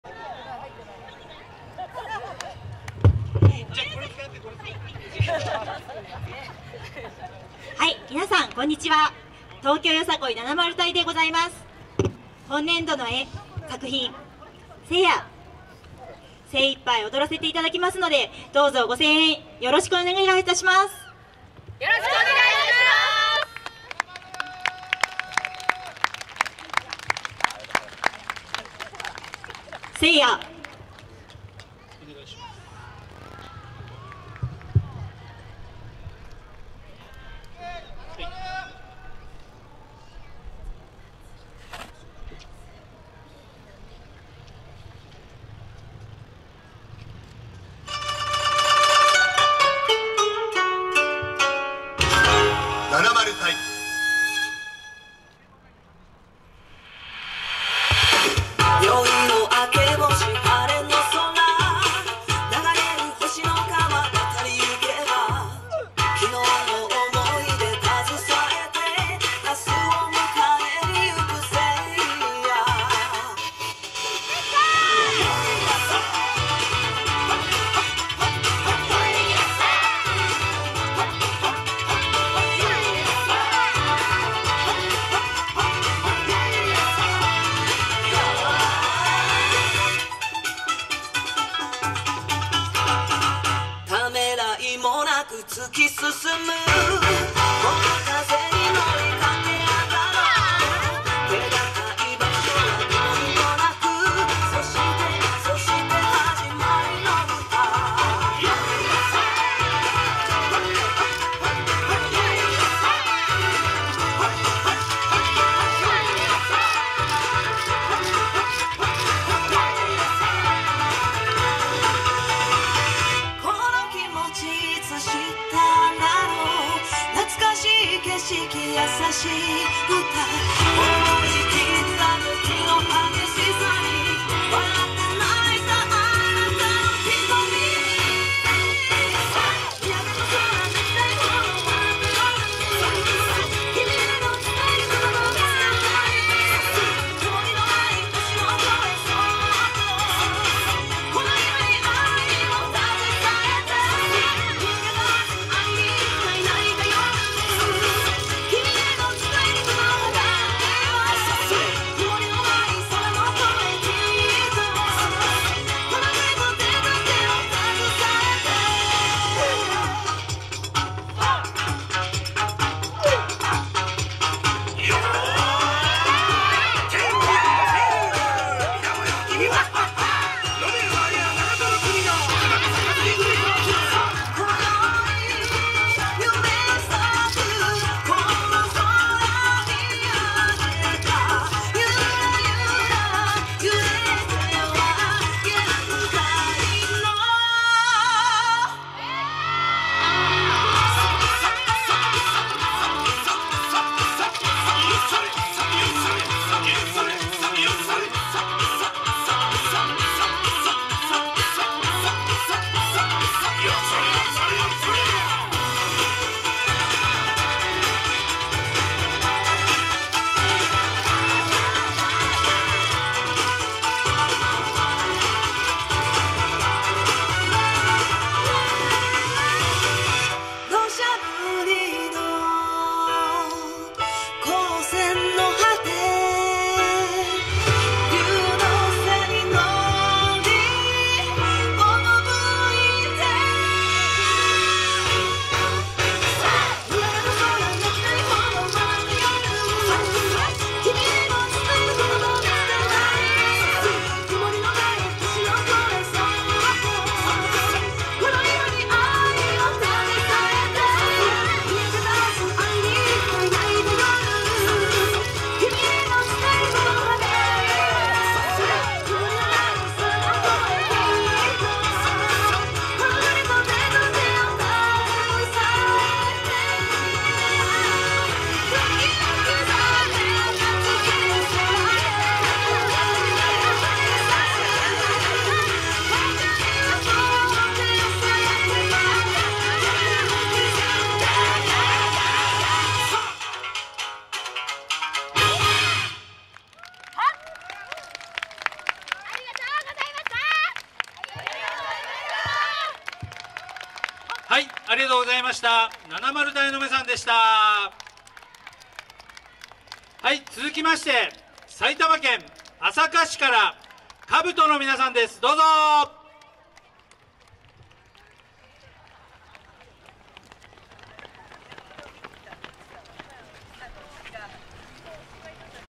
東京よさこい なな丸隊【4K】／ウォーターフロント会場／YOSAKOIそうか2024（第32回草加商工会議所まつり）〜相思草愛〜
2024/11/03（令和6年11月3日）
YOSAKOIそうか2024〜相思草愛〜
【会場】まつばら綾瀬川公園
【主催】草加市よさこい振興会
【共催】草加商工会議所